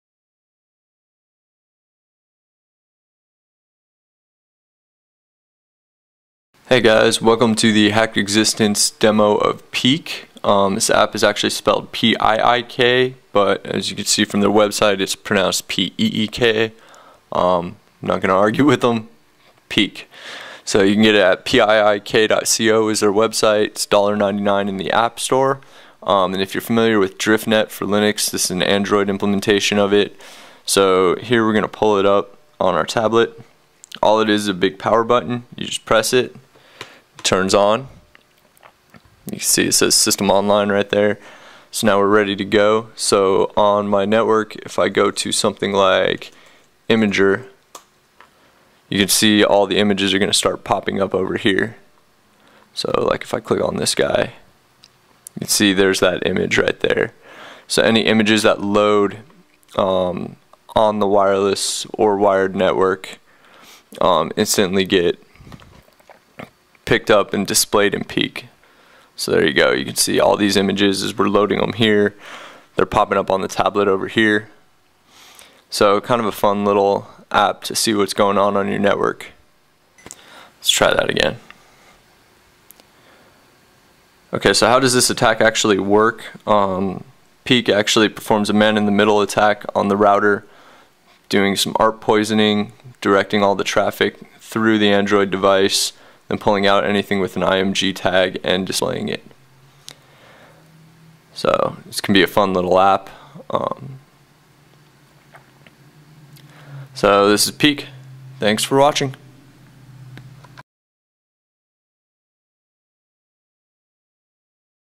Piik - Android Network Traffic Image Extractor
Piik is an Android based Network Traffic Image Extractor.  It works very similar to driftnet on linux.

Piik begins its attack by performing an ARP spoof to redirect all network traffic through the android device.  It then extracts and displays images on the android device in real time as they fly across the network.  When you tap on an image, Piik will show the image source and the host that loaded the image.